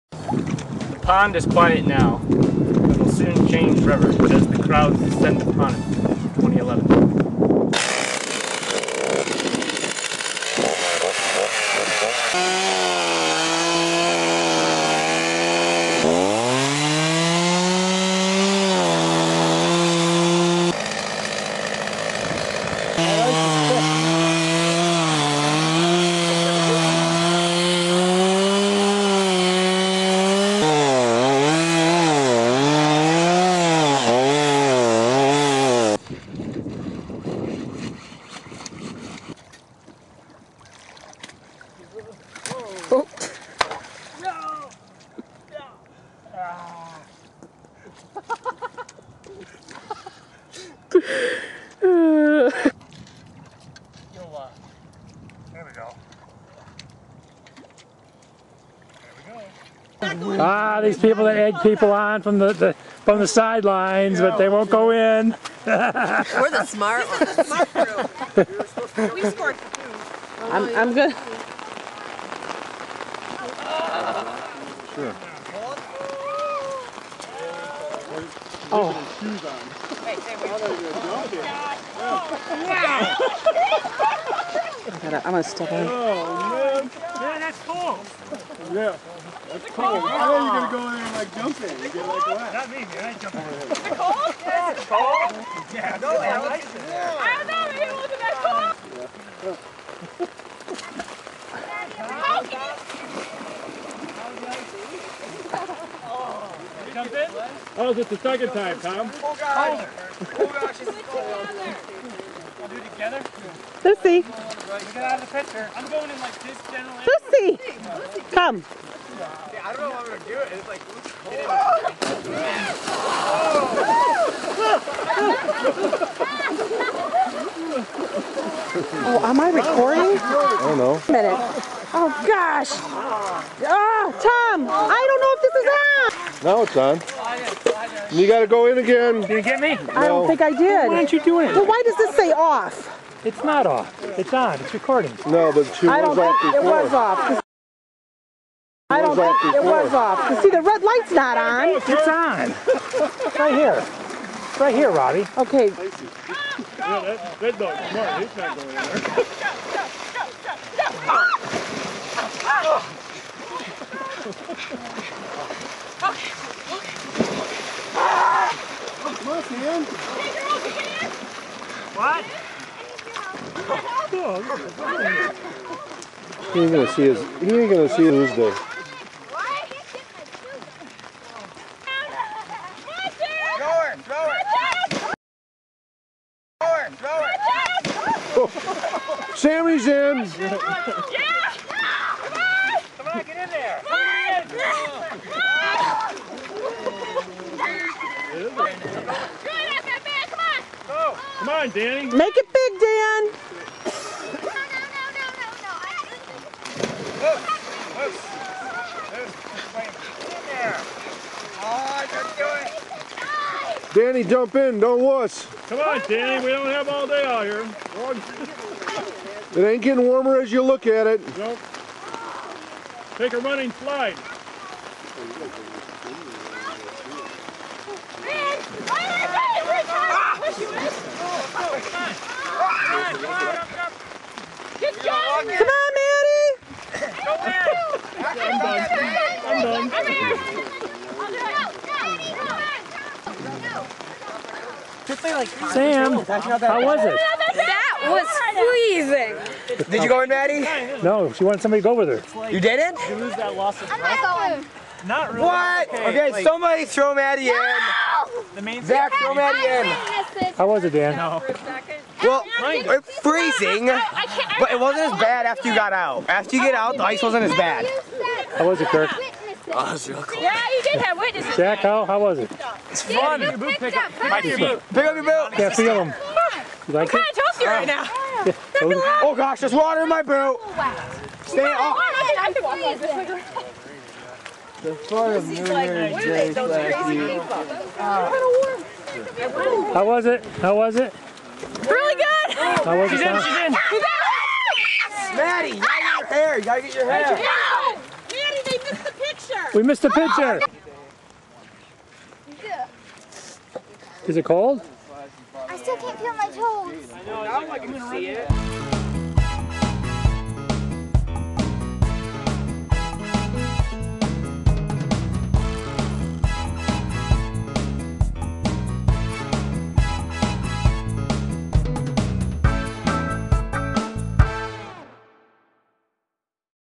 Hartland Polar Bear Club 6th Annual Swim
Our 6th annual swim in our pond that we do each New Year